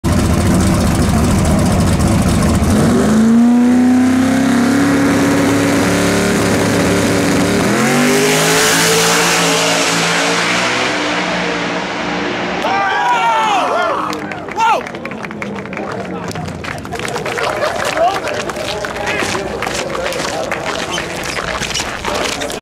EKanooRacing's Super Street RCF sets New Middle East 1/8 Record 4.00@321KM/H (198MPH)
Our Super Street V8 Lexus RC-F reached our initial goal of running 4.00 @ 198 mph during BDRC's Round 3, and it seems that we are moving closer to achieving our ultimate goal of running 3.99 in this car! Can't wait for Round 4!